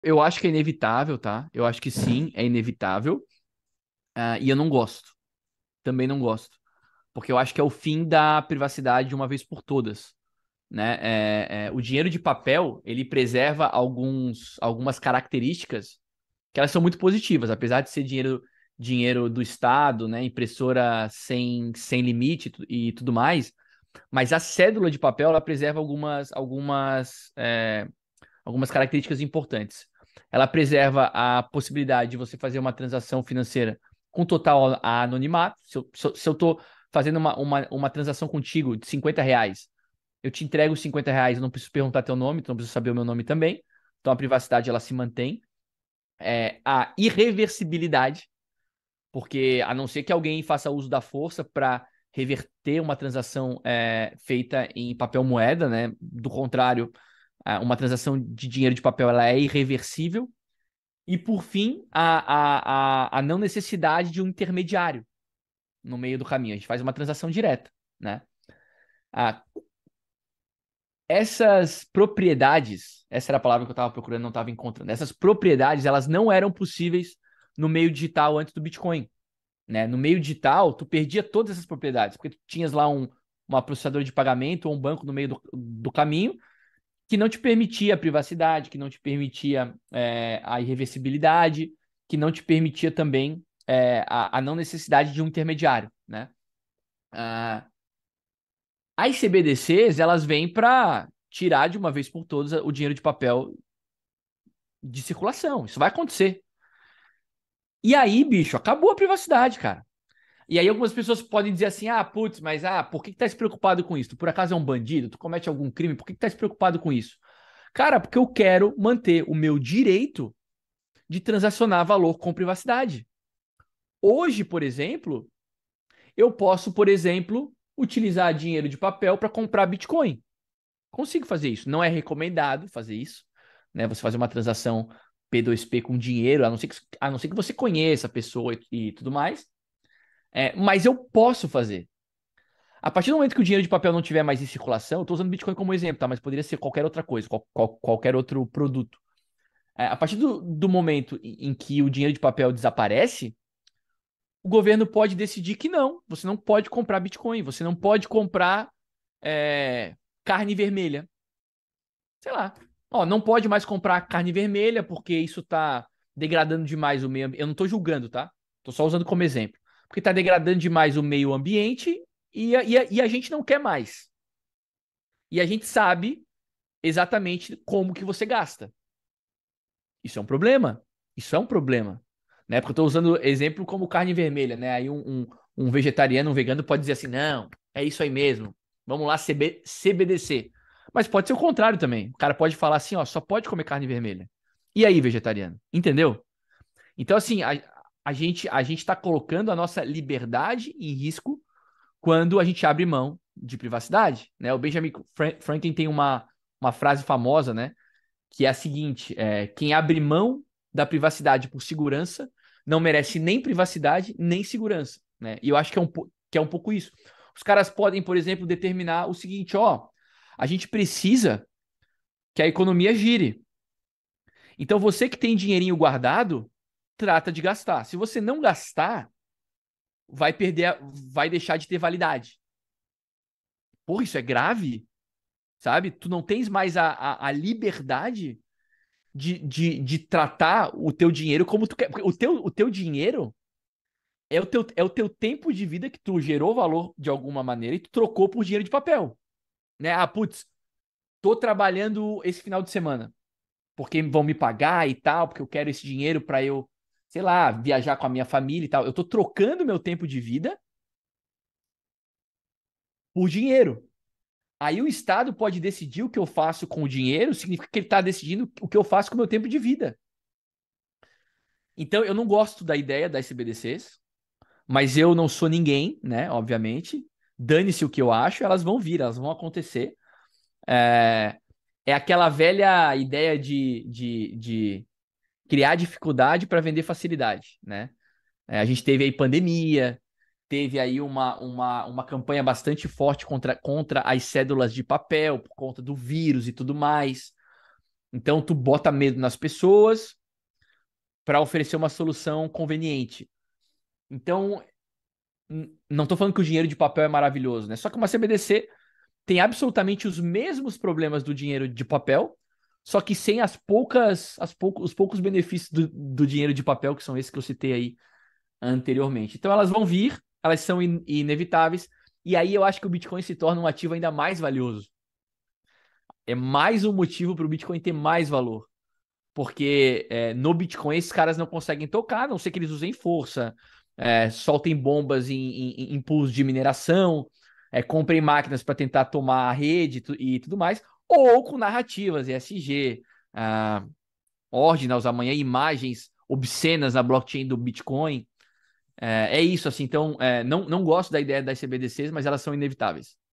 CBDCs São Inevitaveis - João Hazim Explica
João Hazim explica porque CBDCs são inevitaveis

*Confira a legislação do seu país antes de usar a dYdX